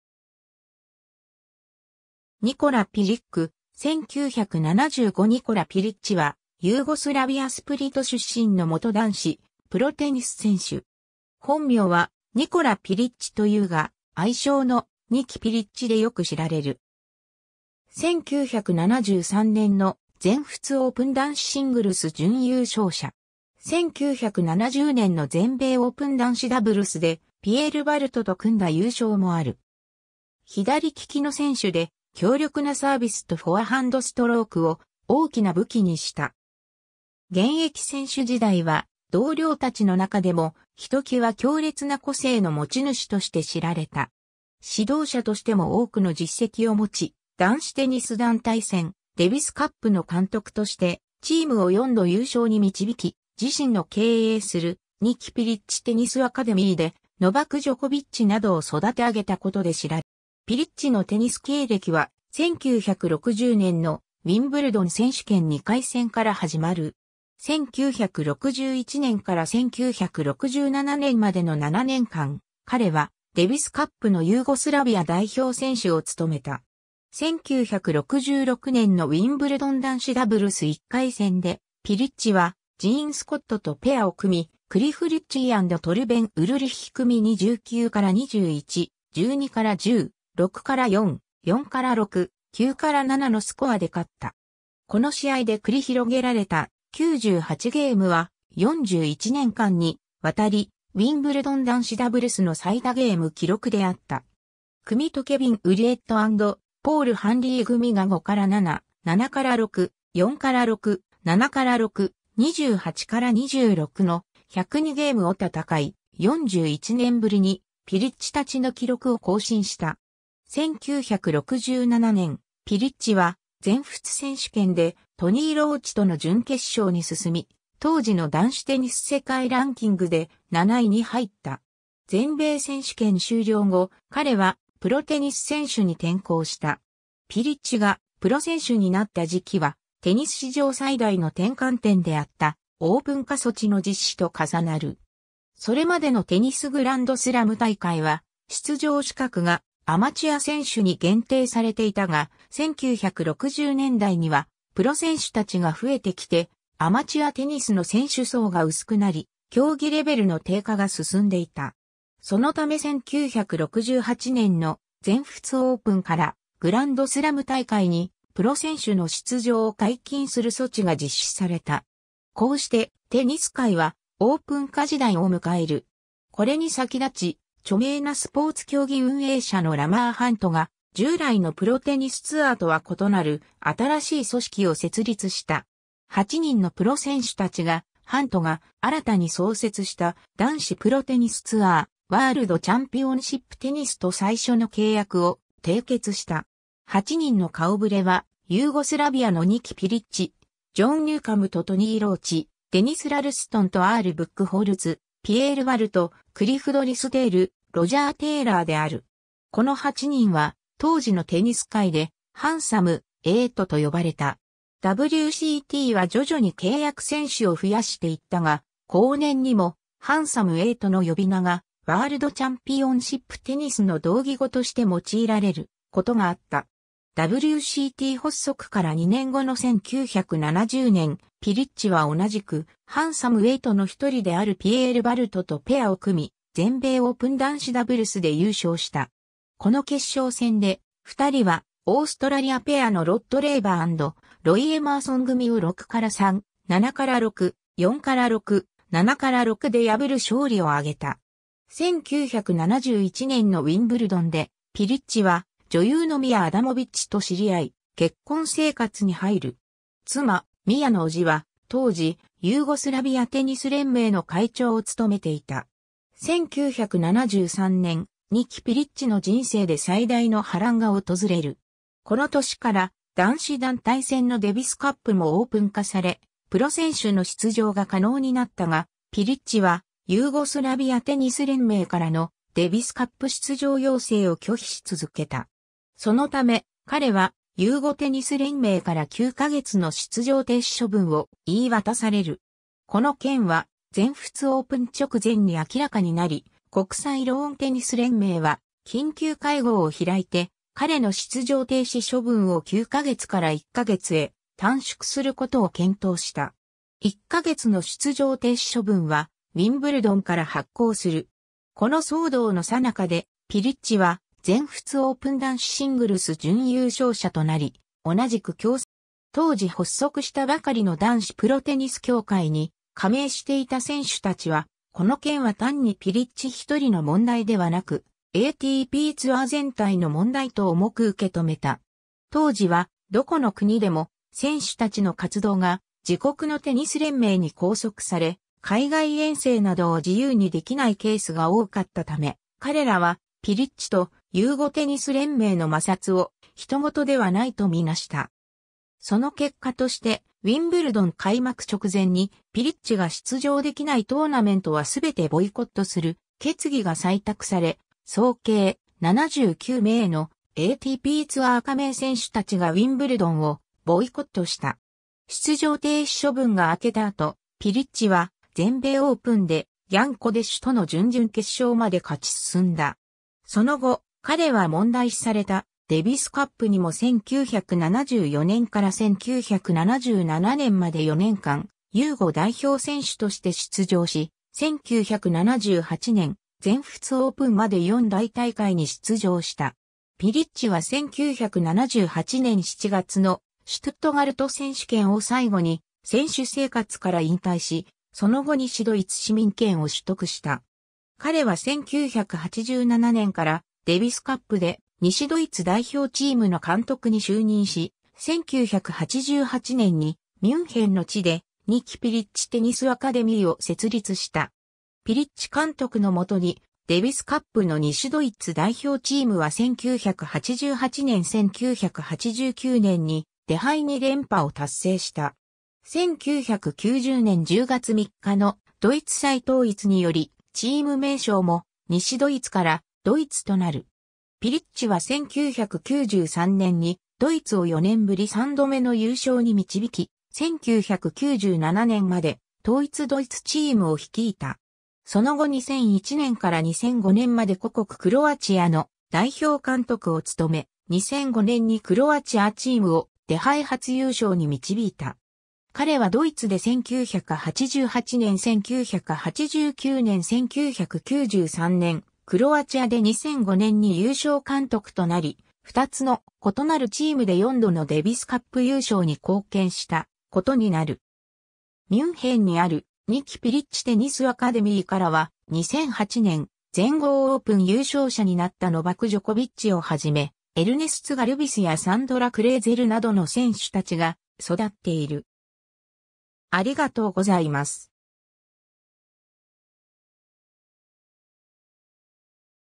ニコラ・ピリッチ
Nikola Pilić, 1975 ニコラ・ピリッチ（Nikola Pilić, 1939年8月27日 - ）は、ユーゴスラビア（現クロアチア）・スプリト出身の元男子プロテニス選手。
本名は「ニコラ・ピリッチ」というが、愛称の「ニキ・ピリッチ」（Niki Pilić または Nikki Pilić）でよく知られる。
1973年の全仏オープン男子シングルス準優勝者。
1970年の全米オープン男子ダブルスでピエール・バルト（フランス）と組んだ優勝もある。
左利きの選手で、強力なサービスとフォアハンド・ストロークを大きな武器にした。
現役選手時代は、同僚たちの中でもひときわ強烈な個性の持ち主として知られた。
指導者としても多くの実績を持ち、男子テニス団体戦・デビスカップの監督としてチームを4度優勝に導き、自身の経営する「ニキ・ピリッチ・テニスアカデミー」でノバク・ジョコビッチ（セルビア）などを育て上げたことで知られる。
 ピリッチのテニス経歴は、1960年のウィンブルドン選手権2回戦から始まる。
1961年から1967年までの7年間、彼はデビスカップのユーゴスラビア代表選手を務めた。
1966年のウィンブルドン男子ダブルス1回戦で、ピリッチはジーン・スコット（アメリカ）とペアを組み、クリフ・リッチー（アメリカ）＆トルベン・ウルリッヒ（デンマーク）組に 19-21, 12-10, 6-4, 4-6, 9-7 のスコアで勝った。
この試合で繰り広げられた「98ゲーム」は、41年間にわたりウィンブルドン男子ダブルスの最多ゲーム記録であった。
（2007年のウィンブルドン男子ダブルス2回戦で、マルセロ・メロ＆アンドレ・サ（ともにブラジル）組とケビン・ウリエット（ジンバブエ）＆ポール・ハンリー（オーストラリア）組が 5-7, 7-6, 4-6, 7-6, 28-26 の「102ゲーム」を戦い、41年ぶりにピリッチたちの記録を更新した。
）1967年、ピリッチは全仏選手権でトニー・ローチ（オーストラリア）との準決勝に進み、当時の男子テニス世界ランキングで「7位」に入った。
全米選手権終了後、彼はプロテニス選手に転向した。
 ピリッチがプロ選手になった時期は、テニス史上最大の転換点であった「オープン化措置」の実施と重なる。
それまでのテニスグランドスラム大会（全豪選手権・全仏選手権・ウィンブルドン選手権・全米選手権）は、出場資格がアマチュア選手に限定されていたが、1960年代にはプロ選手たちが増えてきて、アマチュアテニスの選手層が薄くなり、競技レベルの低下が進んでいた。
そのため1968年の「全仏オープン」から、グランドスラム大会にプロ選手の出場を解禁する措置が実施された。
こうしてテニス界は「オープン化時代」を迎える。
これに先立ち、著名なスポーツ競技運営者のラマー・ハント（1932年 - 2006年）が、従来のプロテニスツアーとは異なる新しい組織を設立した。
8人のプロ選手たちが、ハントが新たに創設した男子プロテニスツアー「ワールド・チャンピオンシップ・テニス」（World Championship Tennis, 略称 WCT）と最初の契約を締結した。
8人の顔触れは、ユーゴスラビアのニキ・ピリッチ、ジョン・ニューカムとトニー・ローチ（この2人はオーストラリア）、デニス・ラルストンとアール・ブックホルツ（この2人はアメリカ）、ピエール・バルト（フランス）、クリフ・ドリスデール（南アフリカ）、ロジャー・テーラー（イギリス）である。
この8人は当時のテニス界で「ハンサム・エイト」（The Handsome Eight）と呼ばれた。
WCTは徐々に契約選手を増やしていったが、後年にも「ハンサム・エイト」の呼び名がワールド・チャンピオンシップ・テニスの同義語として用いられることがあった。
 WCT発足から2年後の1970年、ピリッチは同じく「ハンサム・エイト」の1人であるピエール・バルトとペアを組み、全米オープン男子ダブルスで優勝した。
この決勝戦で、2人はオーストラリアペアのロッド・レーバー＆ロイ・エマーソン組を 6-3, 7-6, 4-6, 7-6 で破る勝利を挙げた。
1971年のウィンブルドンで...